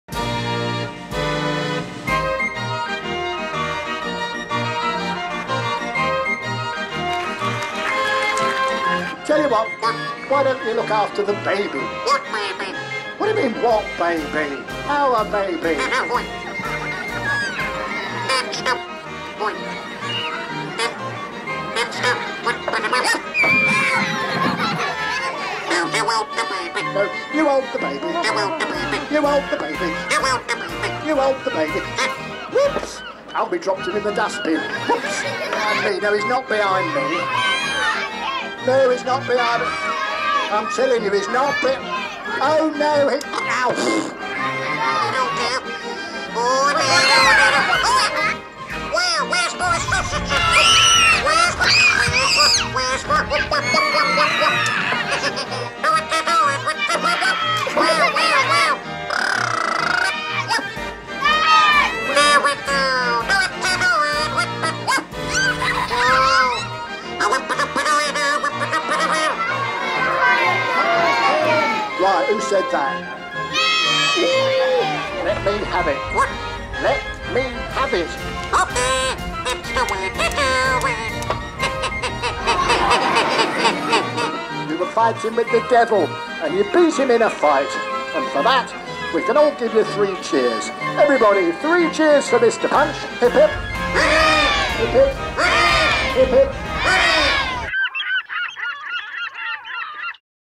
Glyn Edwards Brighton Beach Punch & Judy Show trailer
Short trailer for the full video filmed on Brighton seafront